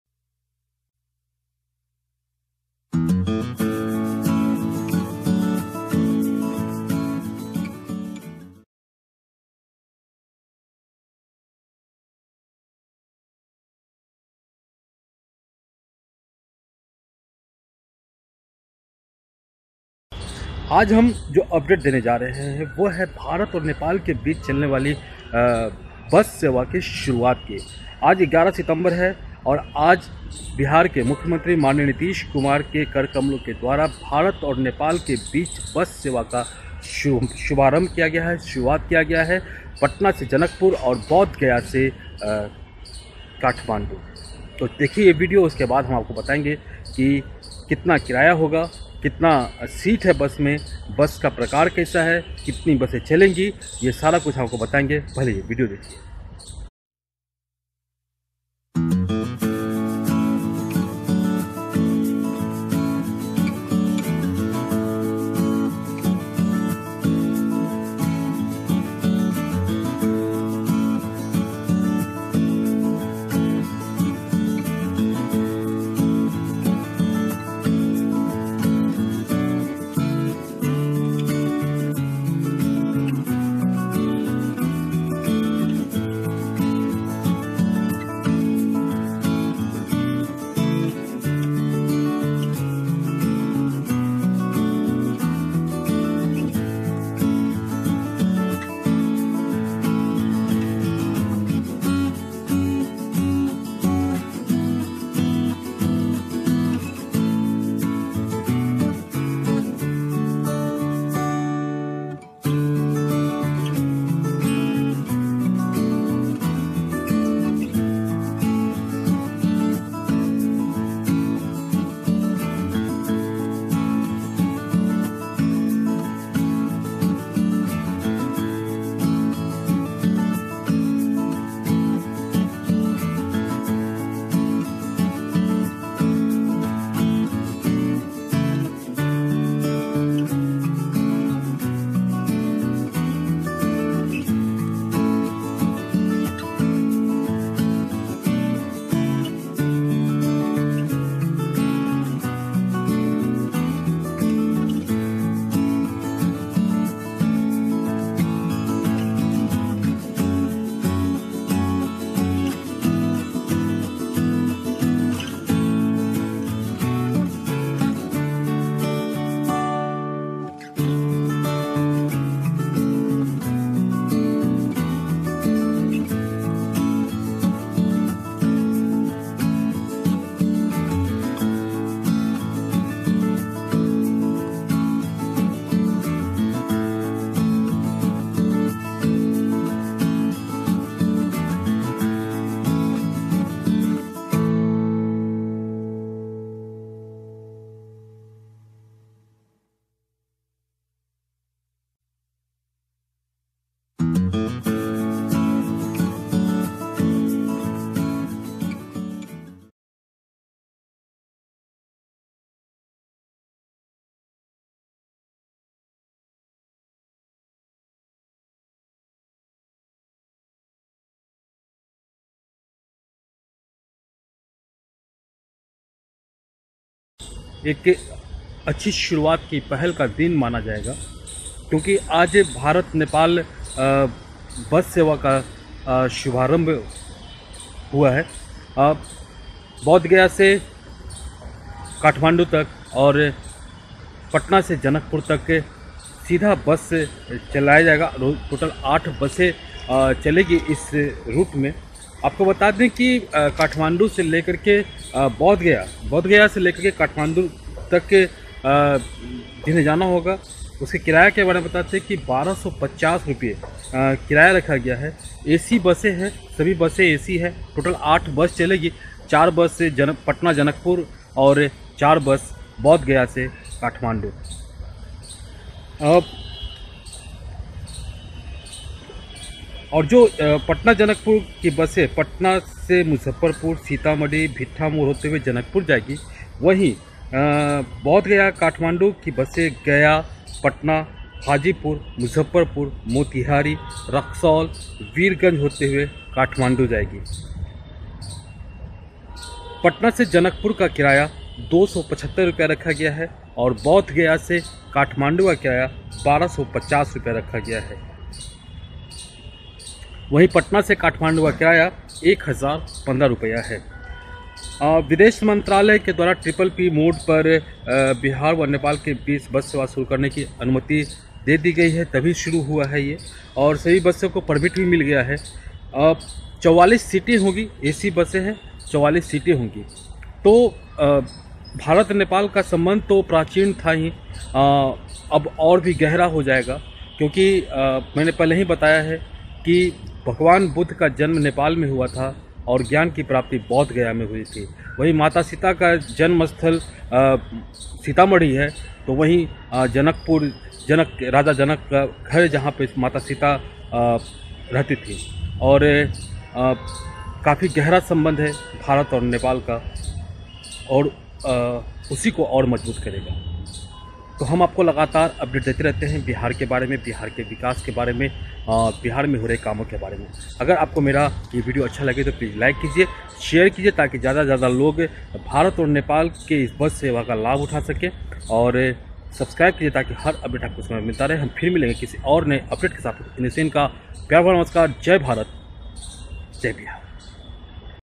Bharat-Nepal Bus Seva start, Flags off By Bihar Chief Minister Nitish kumar on 11 sep 2018
Chief Minister Nitish Kumar on Tuesday flagged off the first bus seva between Bihar and Nepal. After that, Bihar and Nepal are now connected by bus.
This service will link Bodh Gaya and Patna in Bihar to Kathmandu and Janakpur in Nepal. Know about fair n root of this service